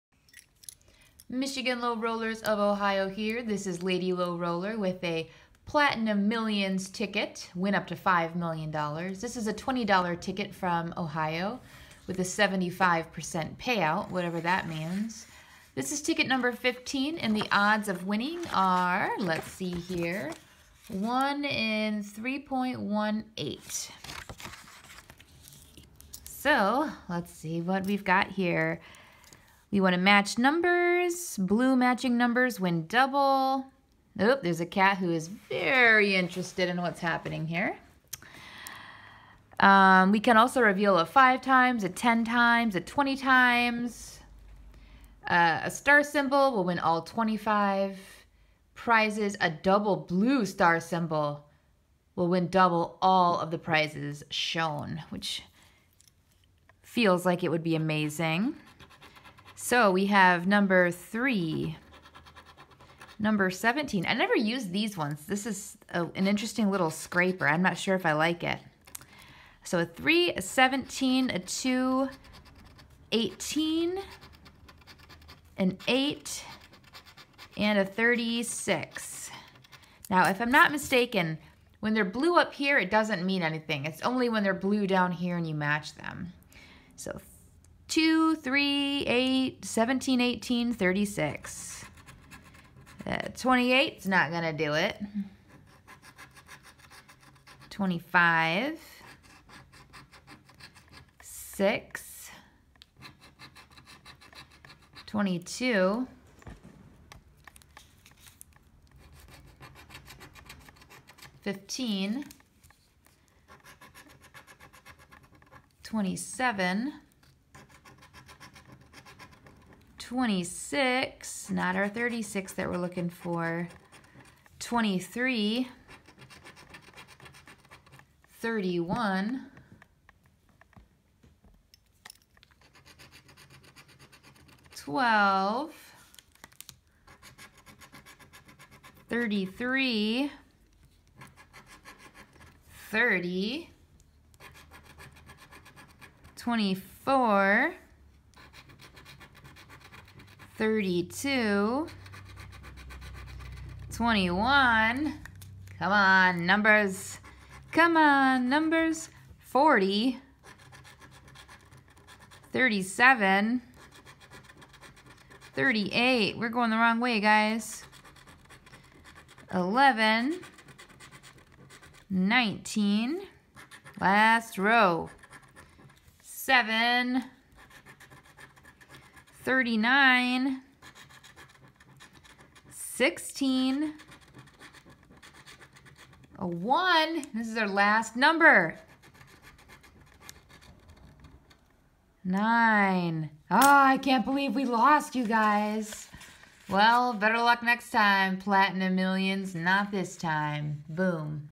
$20 Scratch off Platinum Millions Ohio Lottery ticket!!! BOOOOOM!
Watch as we try to scratch our way to victory. Million dollar winner!!?